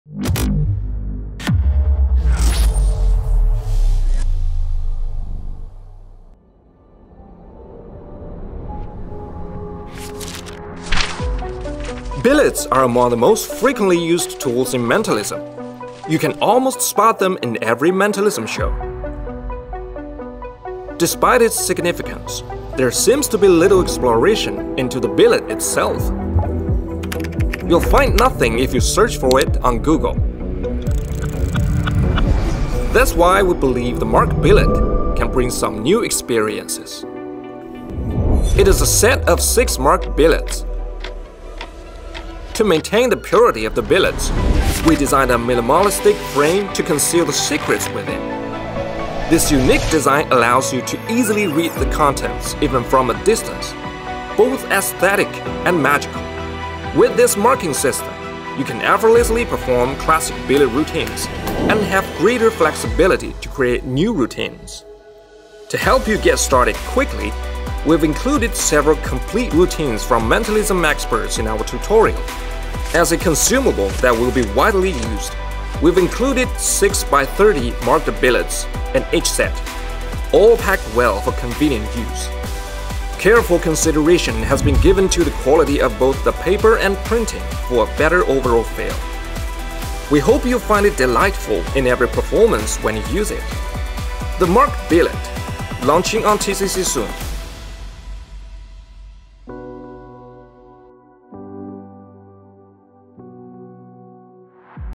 Las papeletas marcadas - TCC ⚡Truco de magia ⚡
✔️ ¡Adelántate a los lanzamientos de TCC en Blackpool con las papeletas marcadas! Este paquete incluye 30 sets de 6 papeletas cada uno, sumando un total de 180 papeletas. Son fáciles de identificar a distancia gracias a su diseño minimalista con secretos en ambos lados. La calidad del papel grueso y la impresión garantizan una escritura estable y un tacto mejorado. Las características principales son las marcas minimalistas, la escritura estable y el tacto mejorado. Cada set tiene seis marcas diferentes. Además, cada juego viene empaquetado individualmente y la caja puede ser utilizada como contenedor. En cuanto al tamaño, cada papeleta mide aproximadamente 9,5 cm × 5 cm. ¡No te pierdas esta oportunidad!